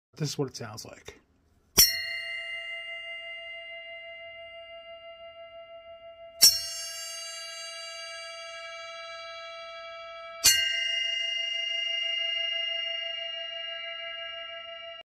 Saluda Cymbals 6" Diamond Series Bell Cymbal
Sound Demo ‎of a Saluda  6" Diamond Series Bell. A "traditional" Bell Cymbal,  similar to a Zildjian Zil-Bell.